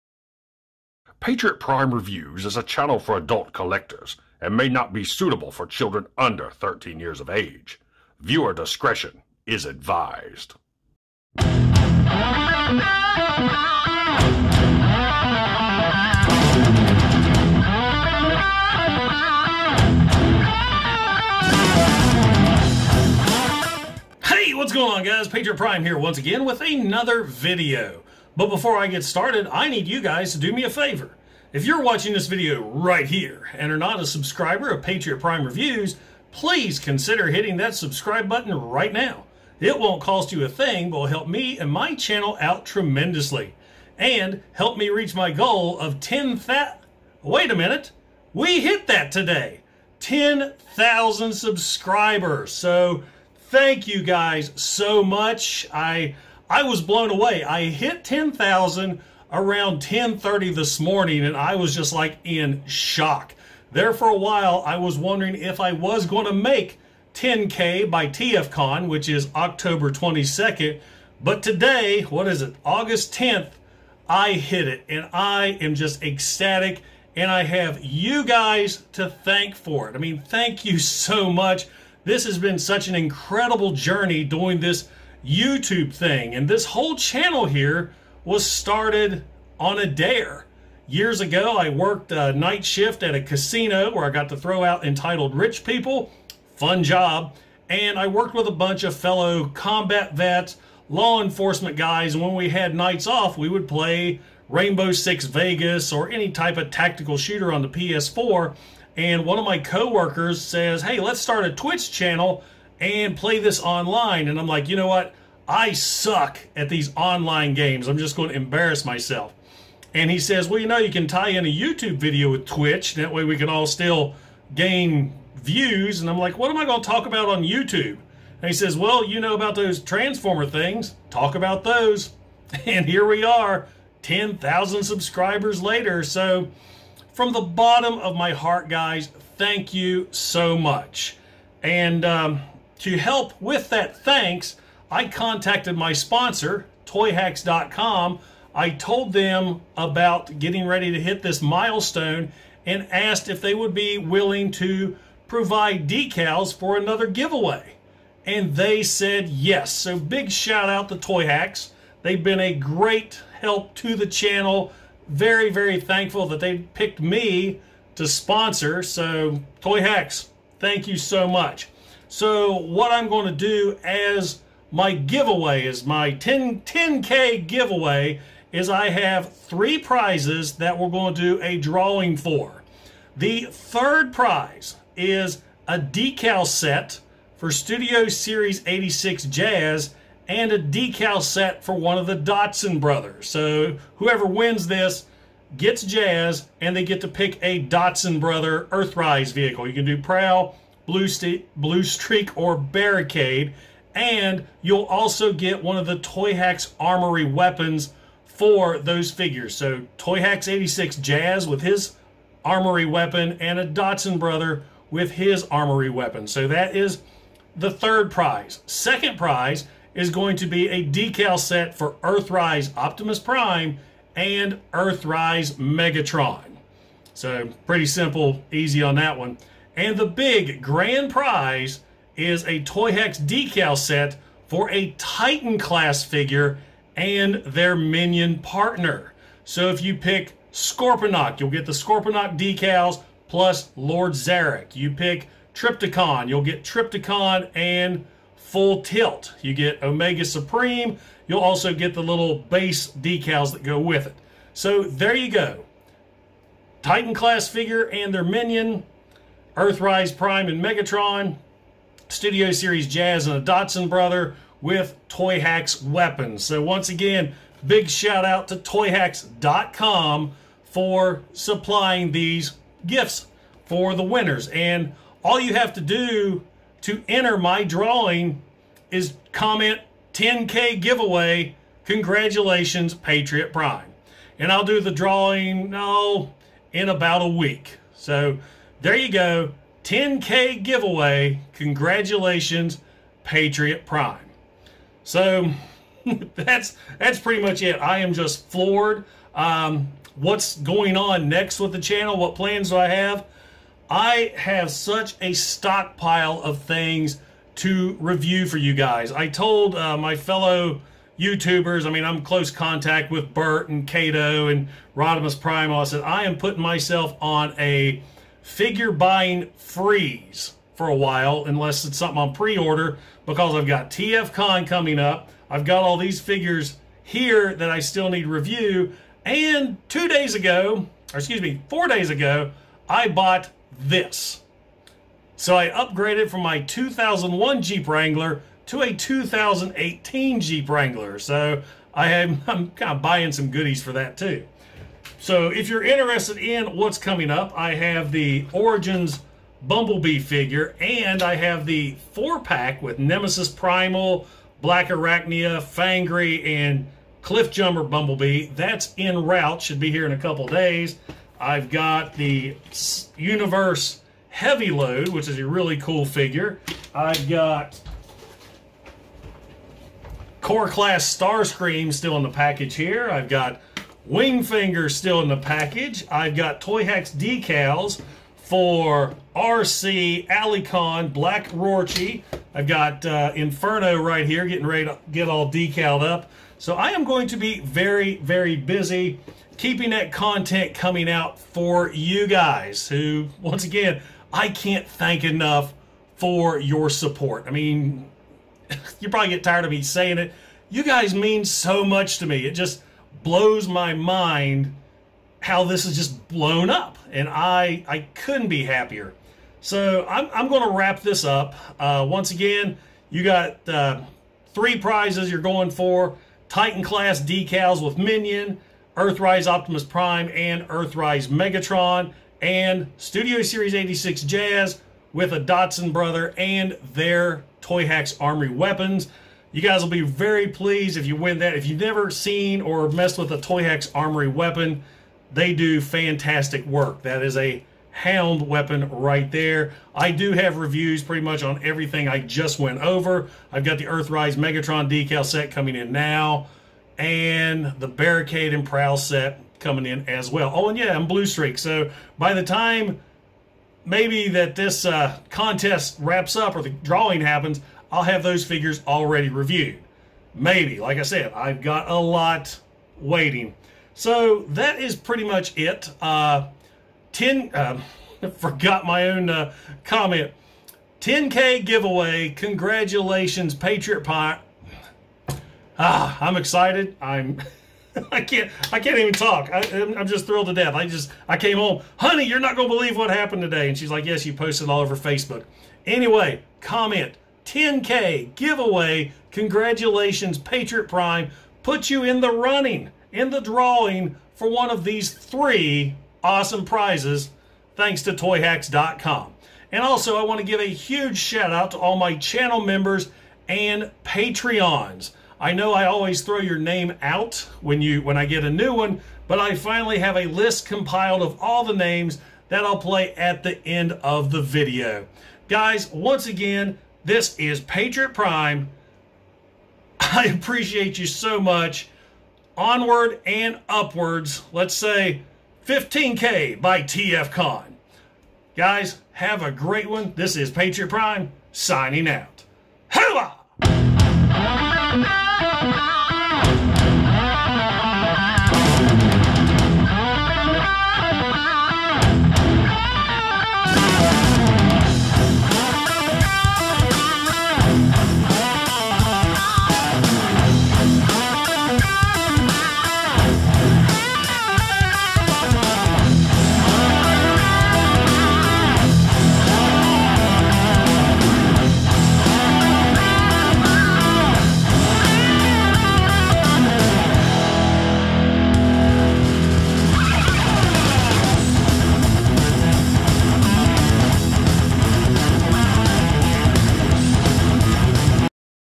Patriot Prime Reviews 10K Giveaway
Mission Accomplished!
"Patriot Prime Reviews" is now a 10K Channel.
Thank you so much for all the support you've shown me since I decided to do this, now onward and upward.
There will be a 1st, 2nd, and 3rd Place Drawing for Toyhax Prize Packs;
1st Prize - Titan Class figure & Minion Decals
2nd Prize - Earthrise Optimus & Megatron Decals
3rd - SS86 Jazz Decals with Armory Rifle & ER Datsun (Prowl, Bluestreak, or Barricade) Decals with Armory Rifle.
Hooah!
***This drawing is for Decals NOT the figures***